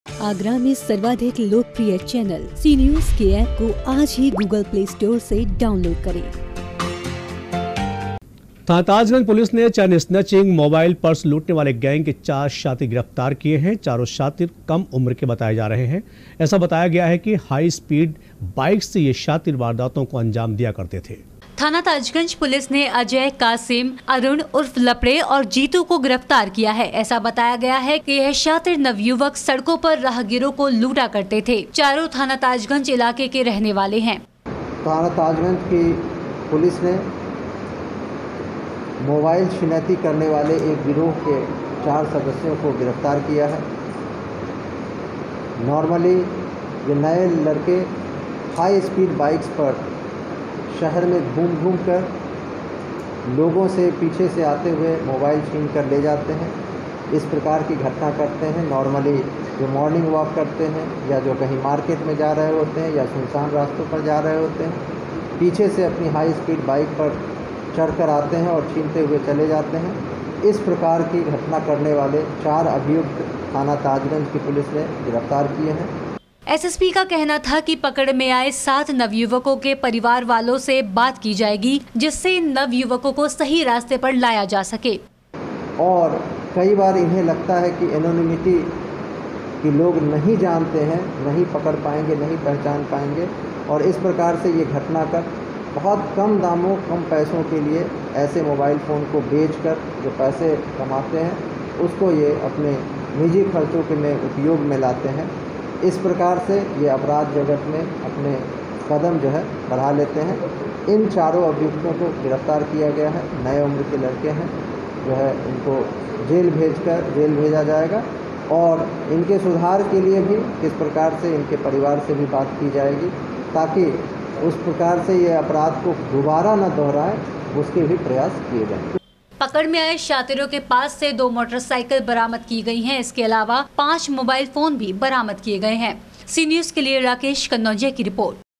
थाना_ताजगंज पुलिस को मिली सफलता, चैन_स्नैचिंग करने वाले गैंग के चार शातिर गिरफ्तार - Sea News Agra
थाना_ताजगंज पुलिस ने चैन_स्नैचिंग, मोबाइल, पर्स लूटने वाले गैंग के चार शातिर गिरफ्तार किए है. चारों शातिर कम उम्र के हैं. ऐसा बताया गया है हाई_स्पीड_बाइक से ये शातिर वारदातों को अंजाम दिया करते थे.